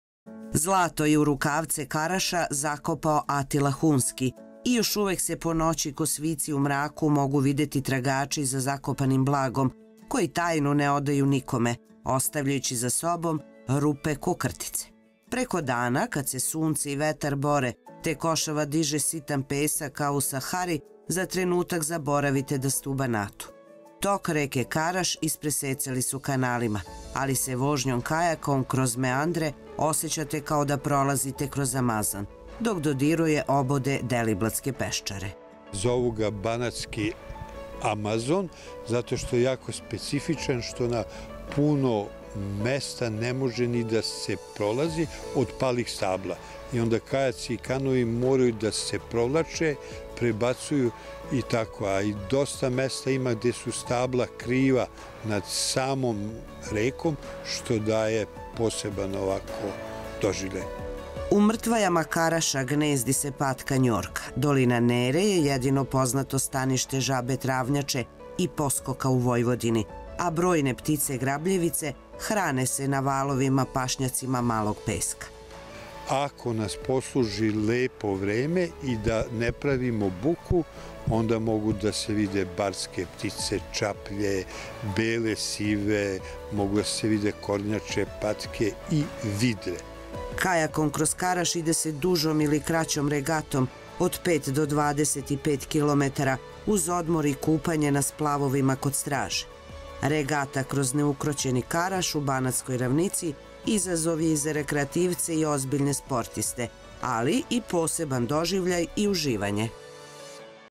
Kajakom kroz Karaš, avantura koju zovu Banatski Amazon
Banatski Amazon je drugi naziv za Karaš, jedinu reku u okolini Vršca, koja se ubraja u predeo izuzetnih odlika. Kajakom kroz Karaš je avantura koju možete da doživite na rečnom toku dužine 60 kilometara, a kako izgleda ta pustolovina, zabeležila je naša dopisnička ekipa iz Banata.